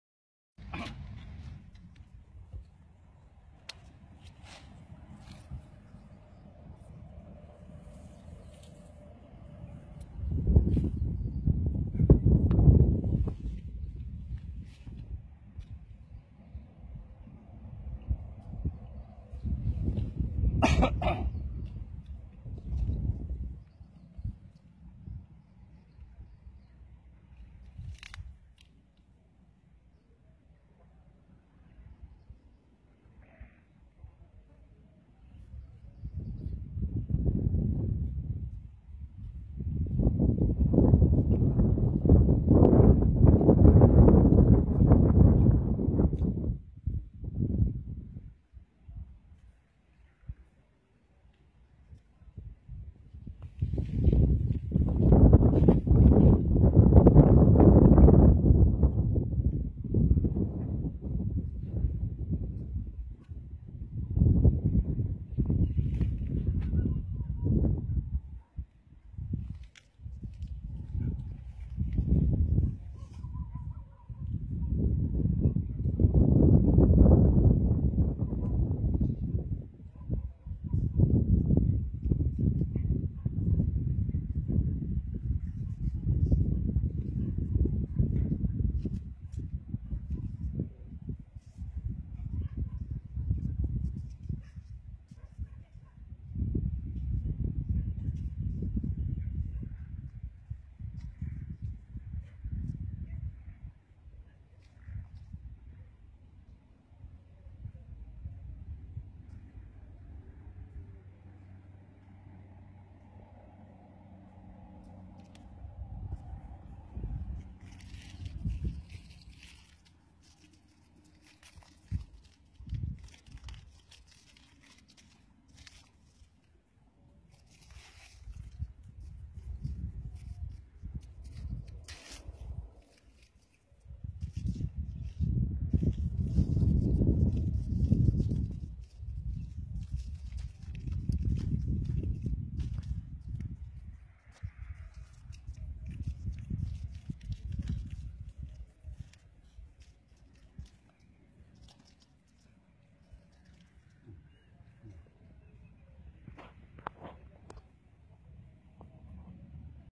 သရက်ပင်ငယ်လေးများ စိန်တလုံးအစေ့ပျိုးပင်ဖြတ်၍ စိန်တလုံးကိုင်းကူးပေးခြင်း သရက်ဒီဇိုင်နာ မြင့်ထွေး
Disigner Myint Htwe